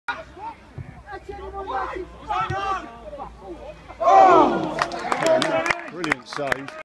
Hitchin Town 1-2 Redditch United - Jonny McNamara Header
Brilliant Jonny McNamara header at goal.